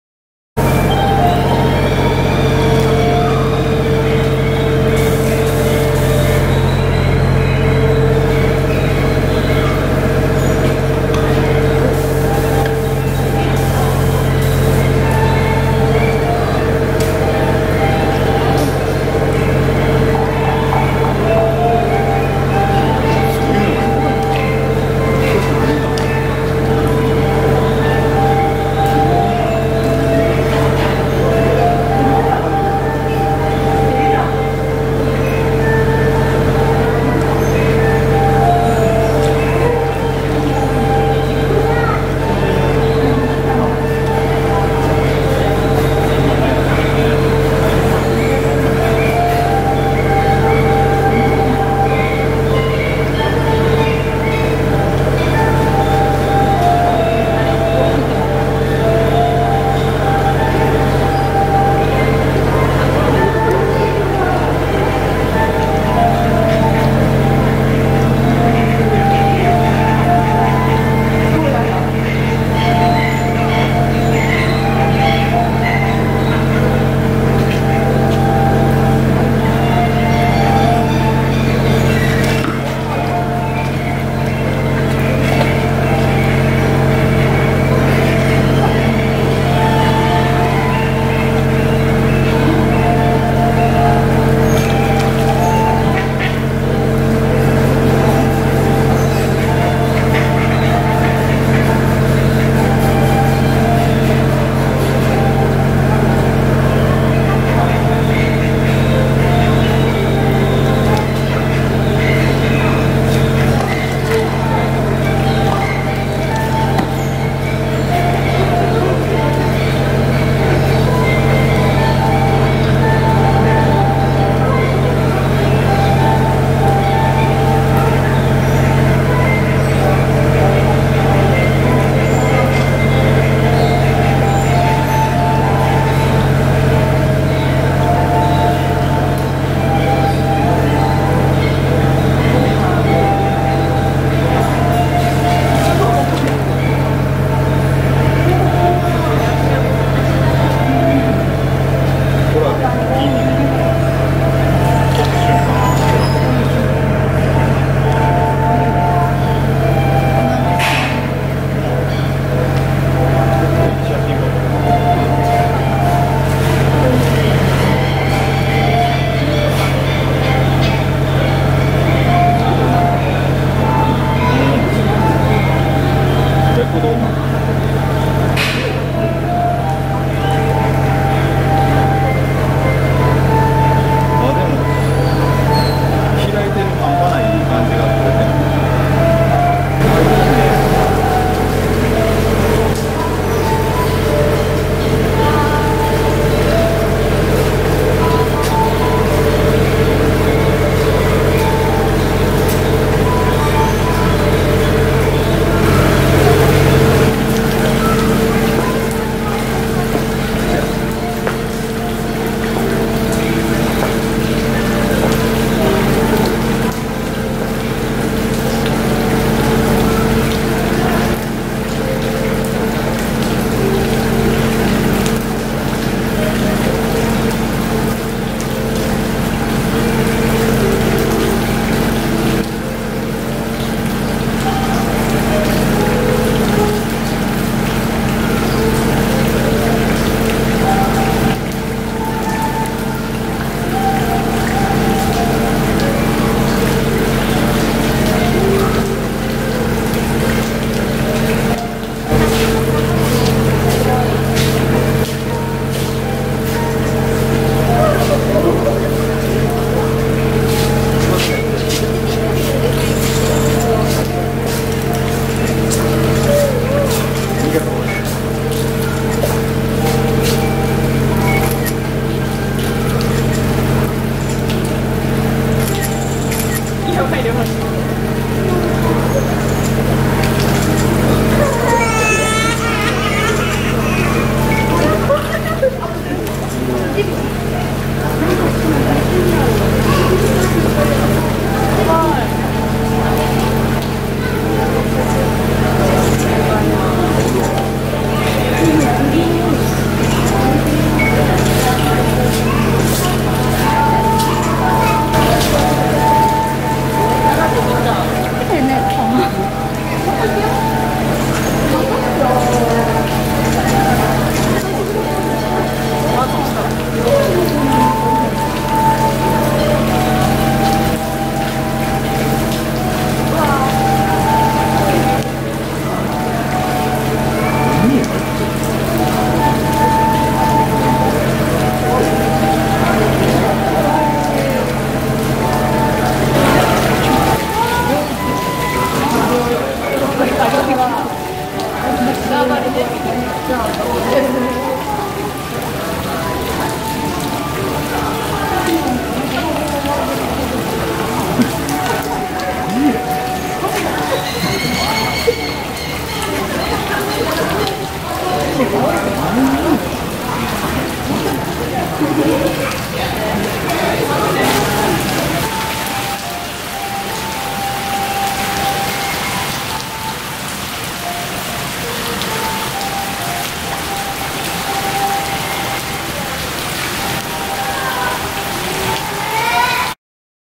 shoebill、雨の中、元気に遊ぶハシビロコウ＠神戸どうぶつ王国
撮影日：2016/09/13  ボンゴ   ペリカンラグーン内で工事が行われているため、不快な音がしています。音量に注意してご覧ください。